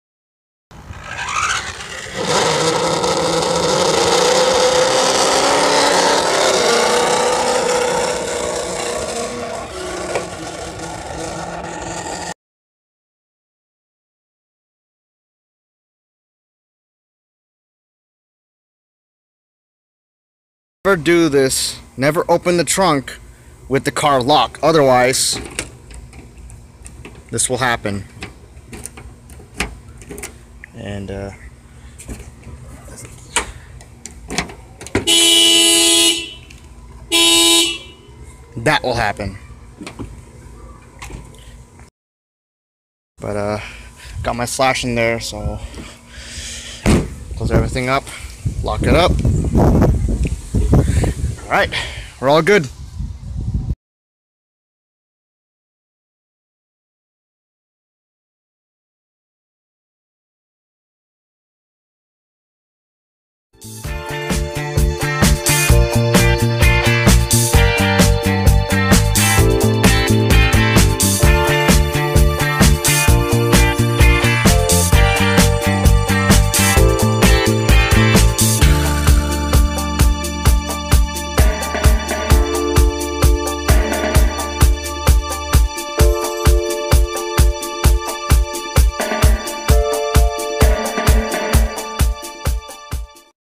Toyota Solara 1st Generation: Never Open the Trunk with the Car Locked
It doesn't sound pleasant

1.  Foul language
2.  Spam
3.  Bullying to other individuals
4.  Racism and sexism. (No sex jokes)